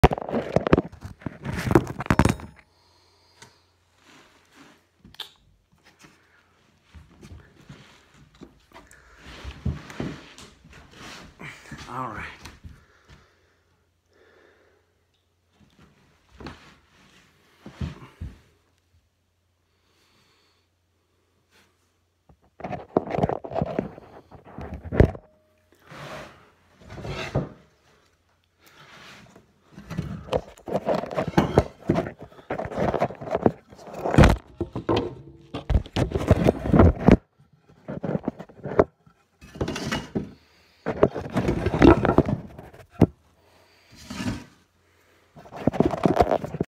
Hump Removal Blooper the level worked and Homer was not ready drop the mic
Nothing like dropping the camera!